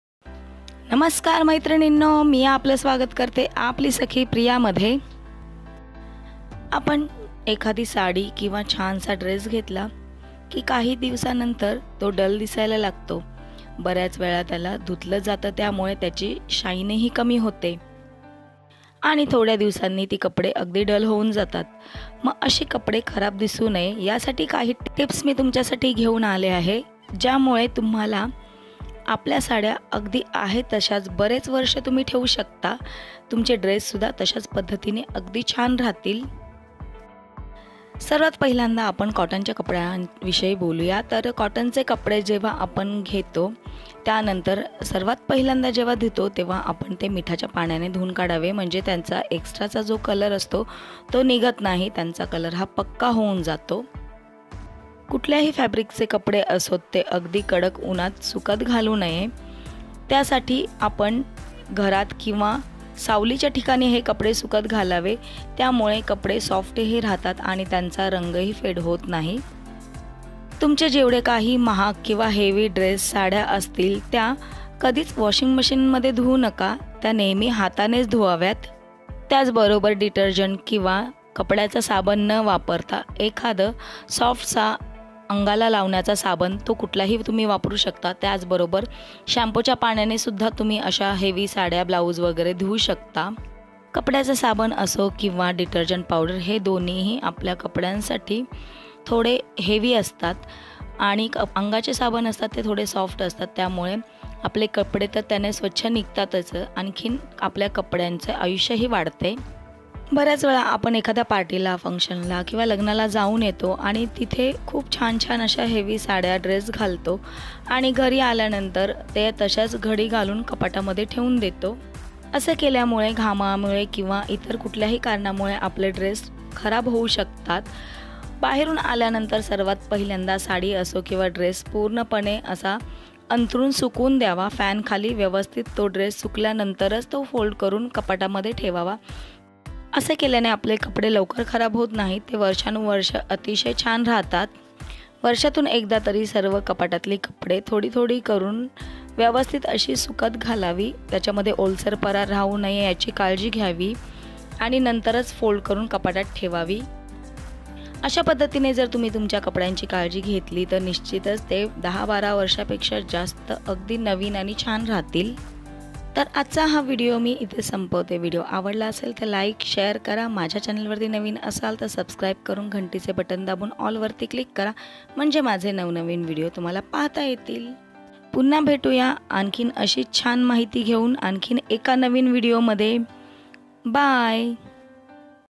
कपड्यांना नव्या सारखं ठेवण्यासाठी च्या काही सोप्या टीप्स/कपड्यांची काळजी कशी घ्यावी/आपली सखी प्रिया
aapli sakhi priya

how to make cloth long lasting

thanks 🙏🙏